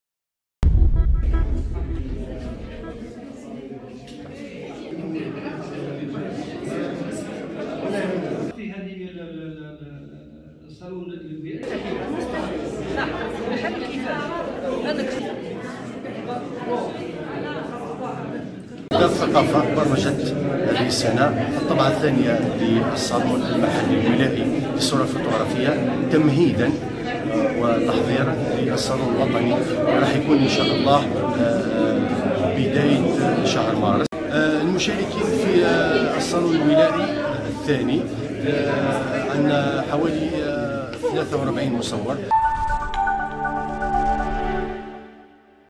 تبسة: مشاركة أزيد من 40 عارضا في الصالون المحلي للصورة الفوتوغرافية
تبسة (وأج)- شارك أزيد من 40 عارضا في الصالون المحلي للصورة الفوتوغرافية في الطبعة الثانية بتبسة‎.